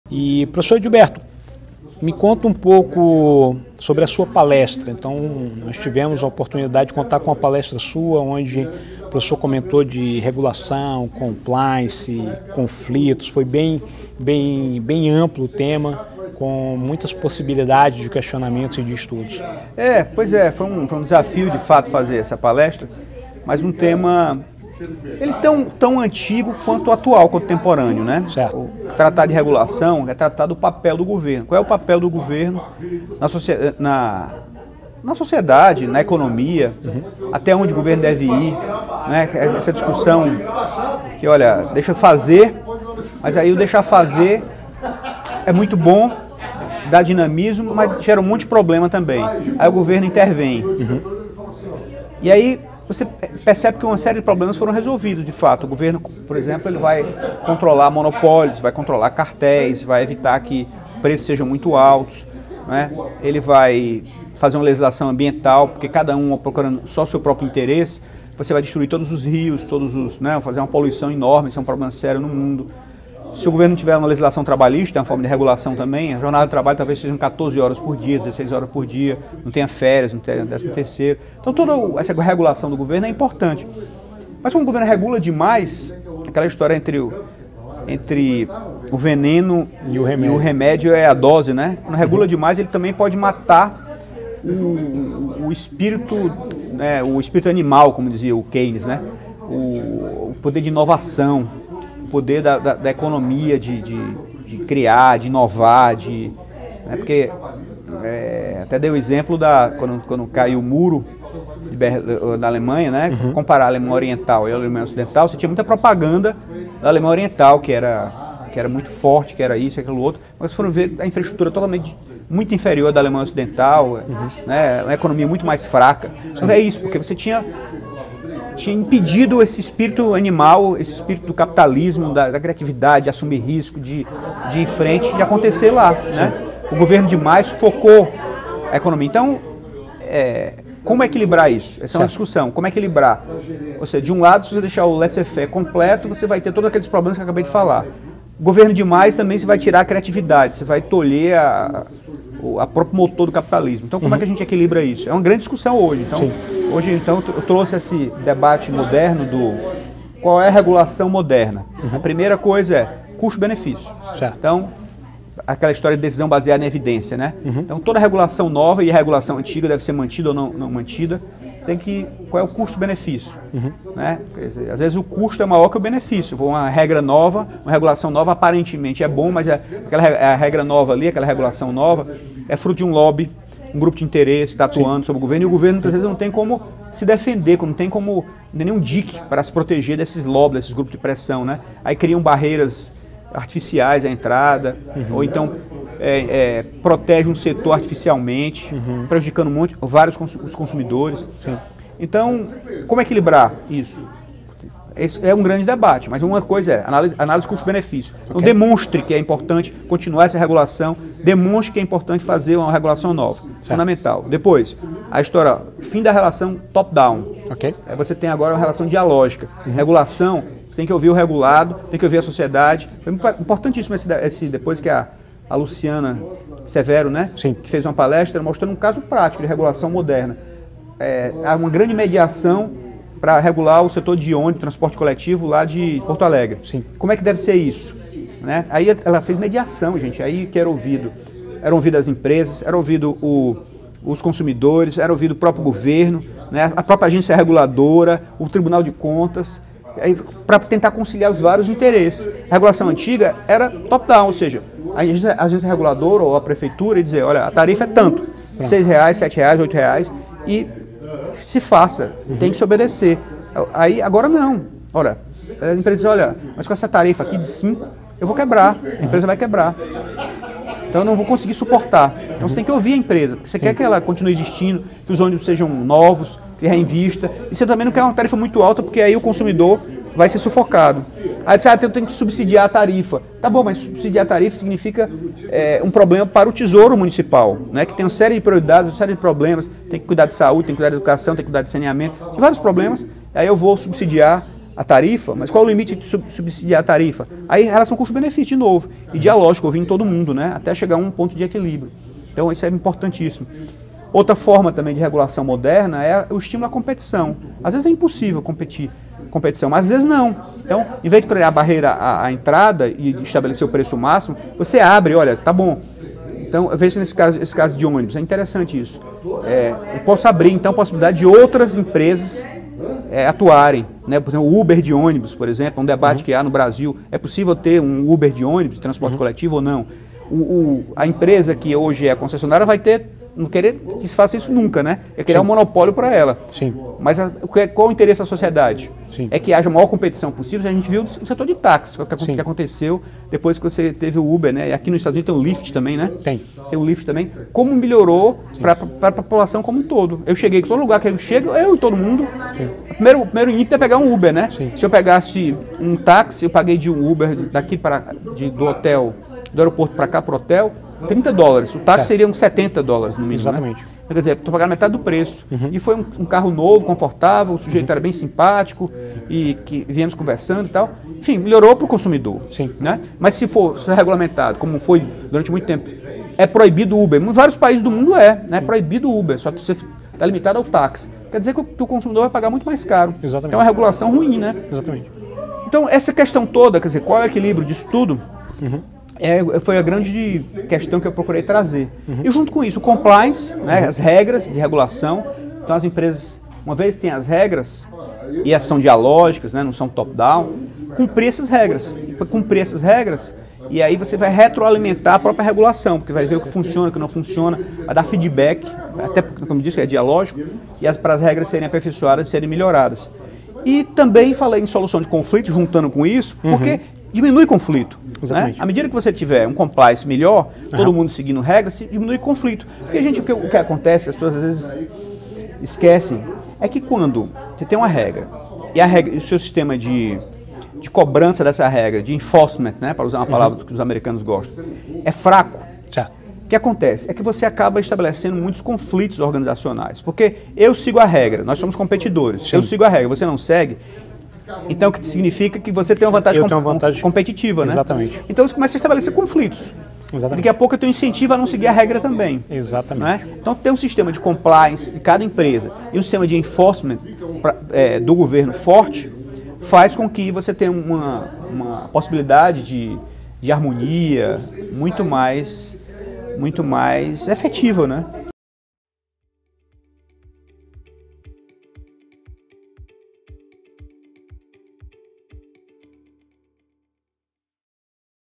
Regulação econômica, compliance e gestão de conflitos organizacionais - Edilberto Lima
Edilberto Pontes Lima, Presidente do IRB, gestão 2024-25, esclarece a importância dos temas abordados em sua palestra “Regulação econômica, compliance e gestão de conflitos organizacionais” na Ambra Conference de 2024.